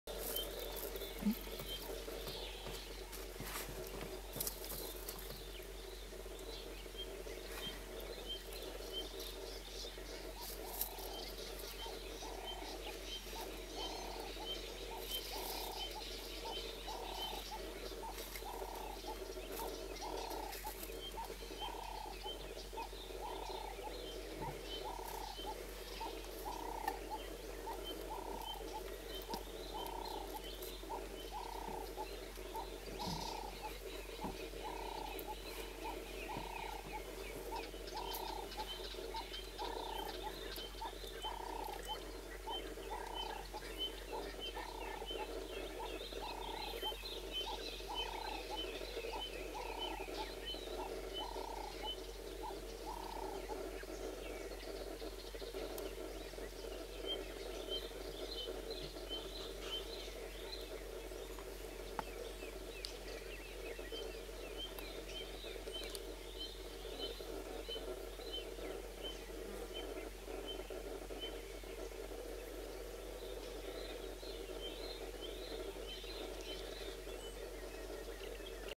Gerda Zandstra MVI 2403
We reden nog maar 5 minuten vanaf de ingang , Matswere gate, toen we onze 1ste leeuwin spotten. CKGR, dec. 2013.